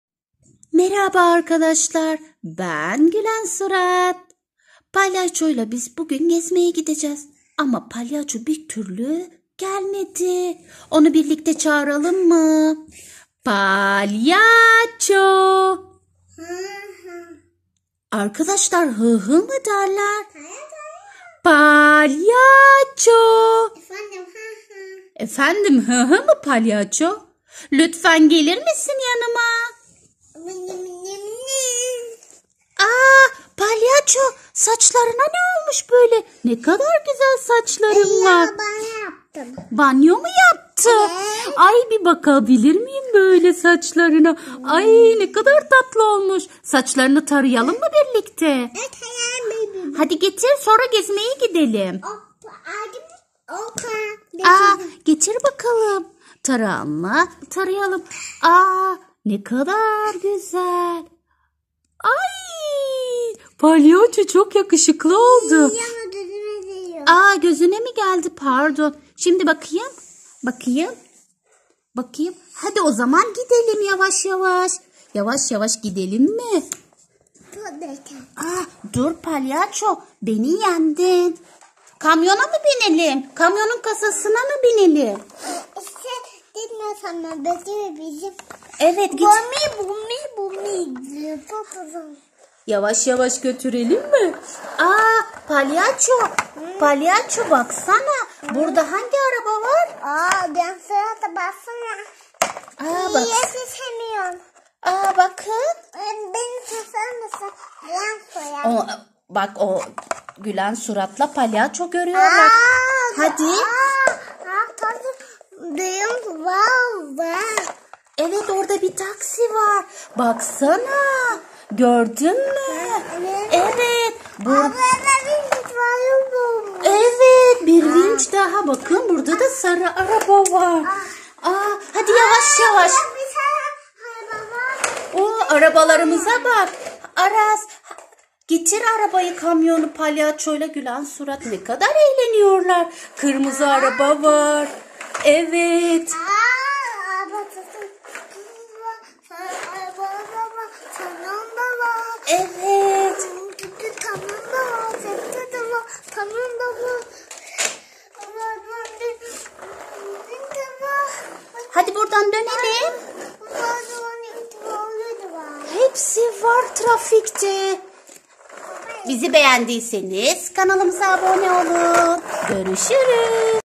çim adam#çim adamlar gülen sürat ve palyaço oyunu
Aras çim adamlarıyla oynadığı çok güzel bir oyun çim adamın saçları dağılmış banyo yapmış onu bir güzel taradı sonra oyuncak kamyonla gezmeye gittiler  bizi izleyin🤩(Aras zuzi)